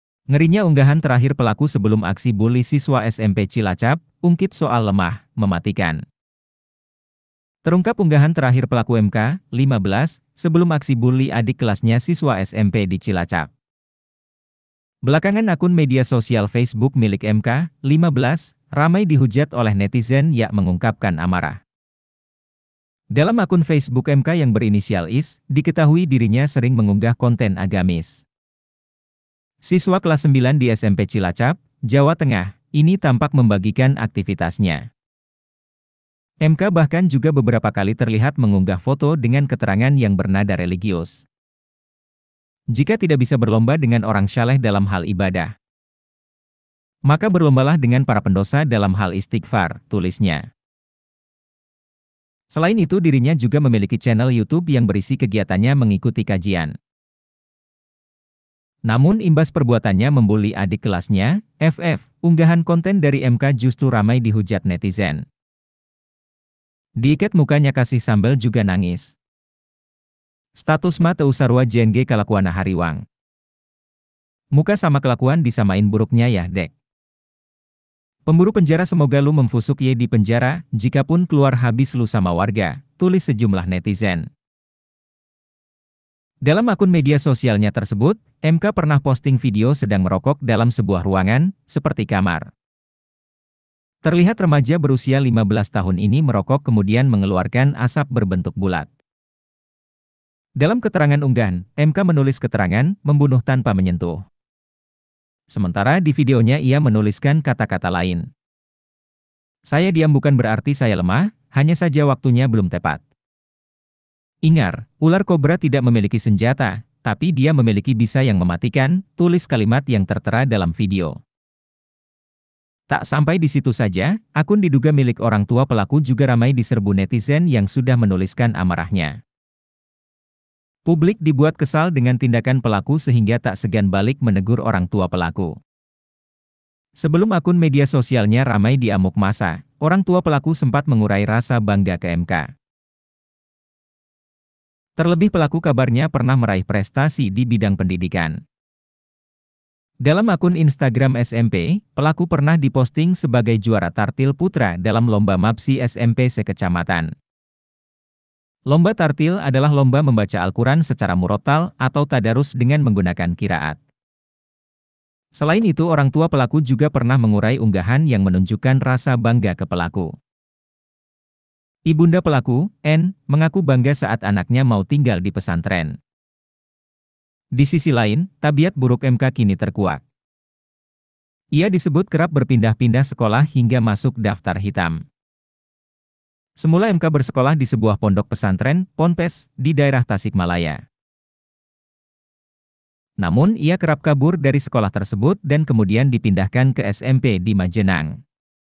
Ngerinya Unggahan Terakhir Pelaku sebelum Aksi Bully Siswa SMP Cilacap, Ungkit Soal Lemah: Mematikan
Terungkap unggahan terakhir pelaku MK (15) sebelum aksi bully adik kelasnya siswa SMP di Cilacap.